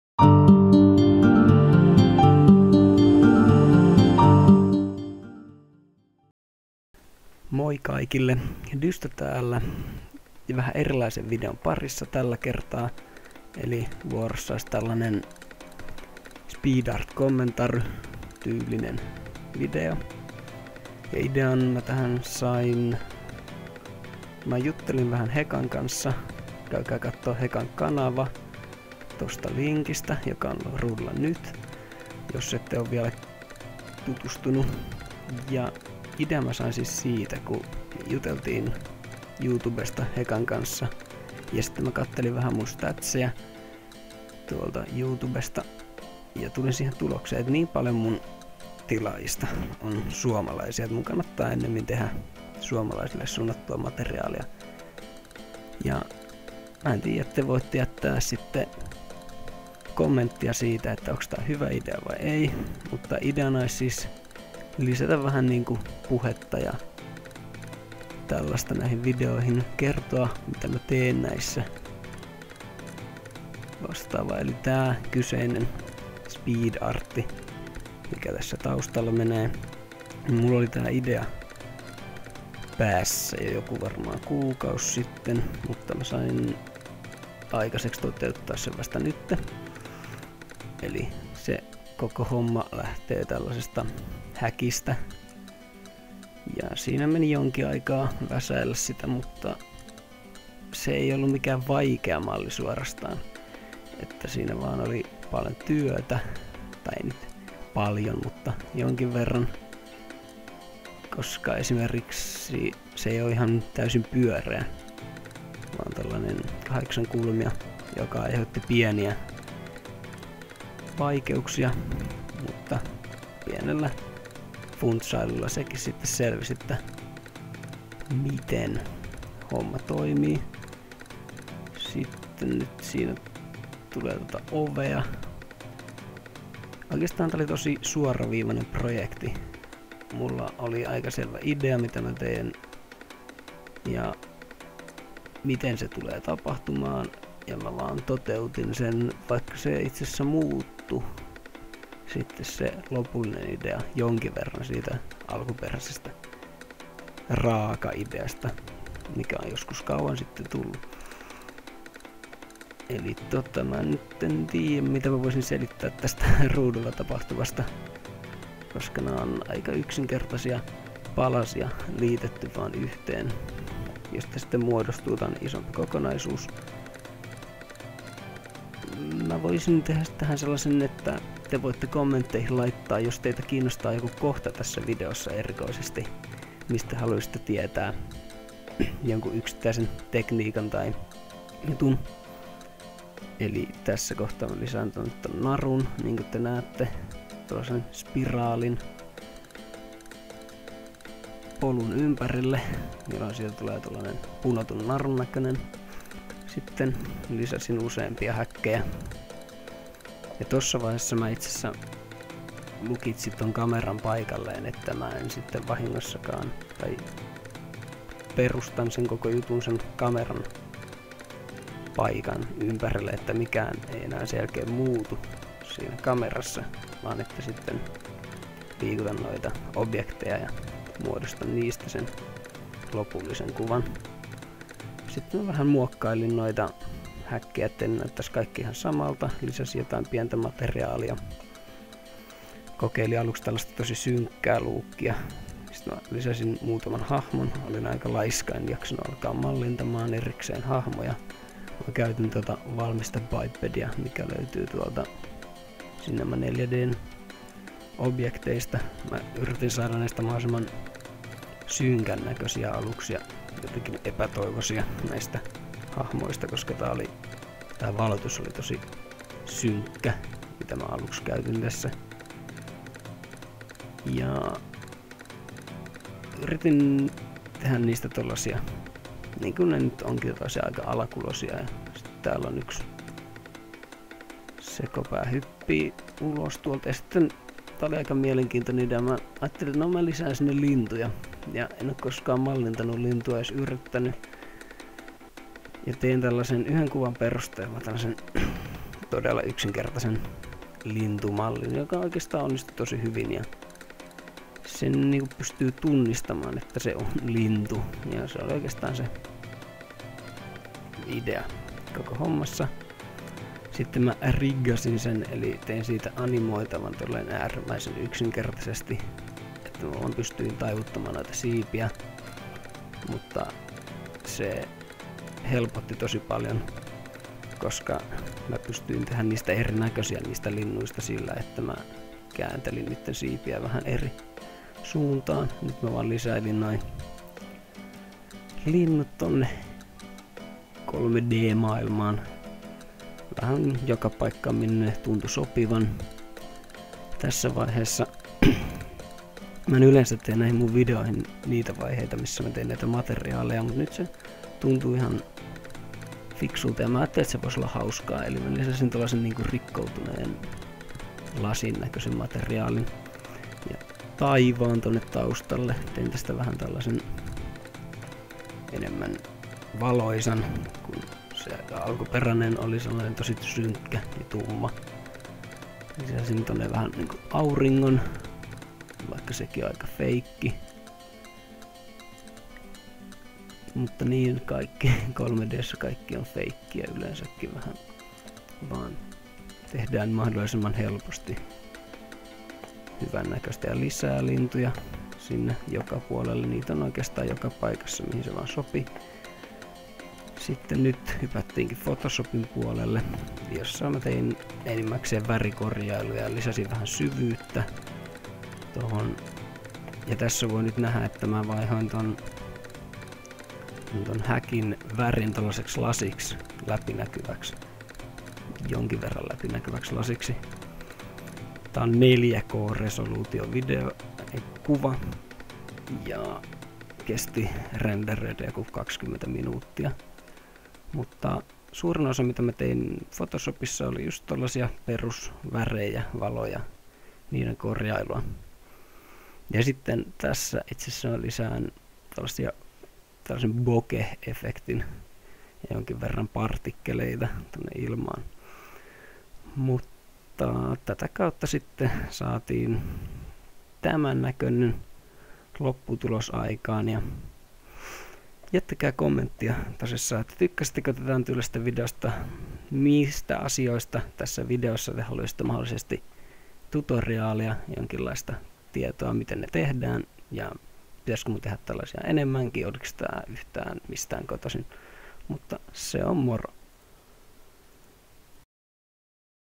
Aamunkoitto | Speed Art | Suomi Kommentary
Avaa/Open

Suomeksi:

Kokeilin hieman erilaista ja toivottavasti mielenkiintoisempaa tapaa tehdä speed art videoita. Ideana on siis että selitän videon aikana mitä teen ja miksi niin teen.

Jätä kommentti mitä mieltä olet ideasta ja kannattaako sitä viedä eteenpäin. Voit myös toivoa tutoriaalia jostakin videossa esiintyvistä tekniikasta yms.

Jos tykkäsit videosta tykkää siitä myös painamalla tykkää nappia

oikea aika: noin 6 tuntia

käytetyt ohjelmat: Cinema 4D ja Photoshop

kannattaa myös tykätä facebook sivustani koska sinne tulee paljon sellaista materiaalia jota et youtubessa näe

English: This is my last speedart: Aamunkoitto(daylight)

programs used: Cinema4D and Photoshop